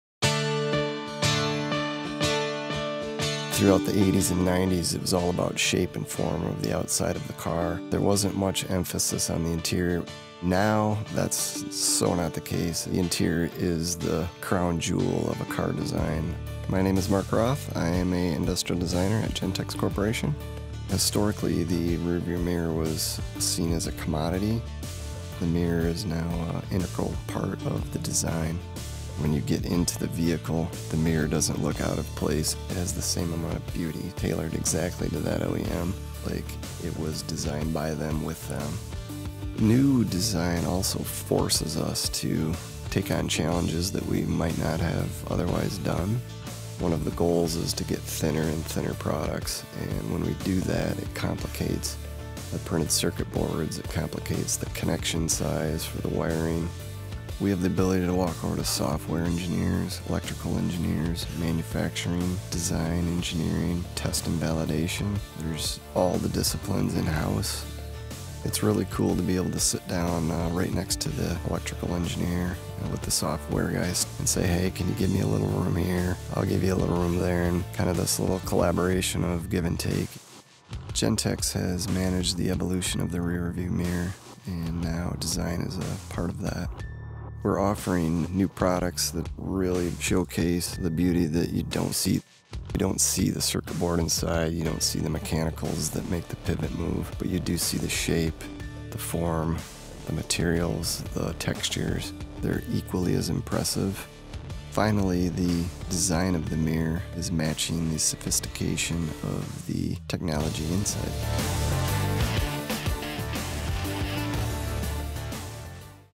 Advanced Mirror Styling - Designing in the Automotive Industry
Throughout the 80's and 90's, the majority of the focus in automotive design was on the form and the outside of the car. Not much attention was paid to the interior. That's no longer the case today, and many automotive companies have started putting serious thought into what goes inside their vehicles. Gentex has remained at the forefront of that charge by developing advanced mirrors with unique features and designs for each of our customers. Finally, the design of the mirror is matching the sophistication of the technology inside.